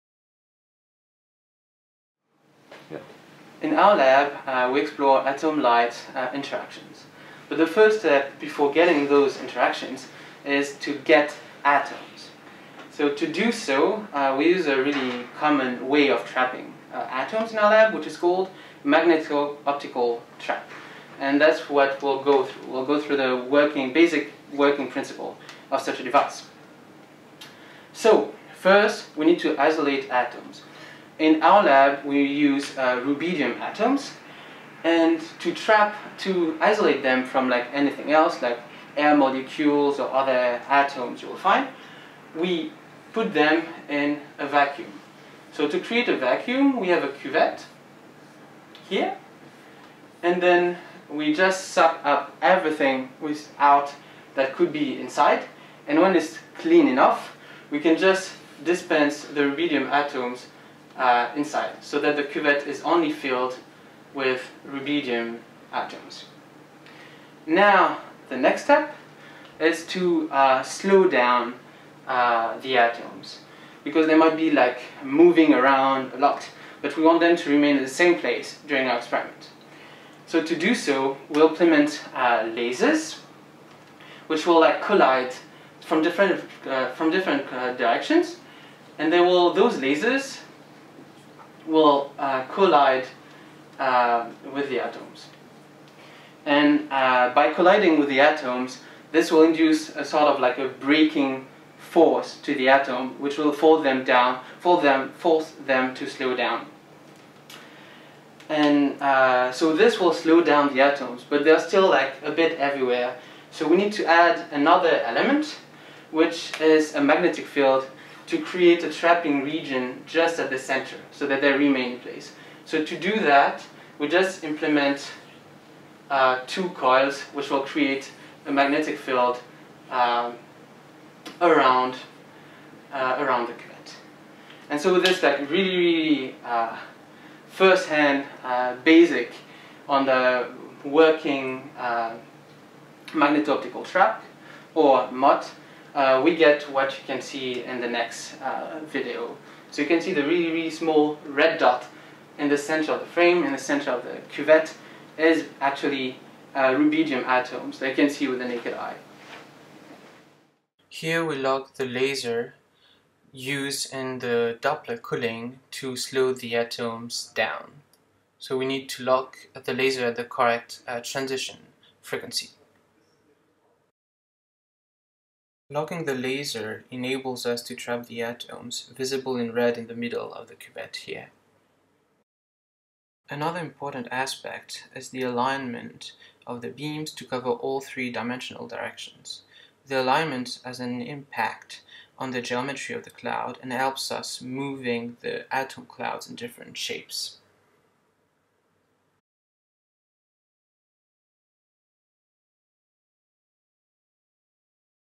How to cool and trap rubidium atoms with a laser
Florentin shows how he prepares a cloud of very cold atoms in a so-callend magneto-optical trap.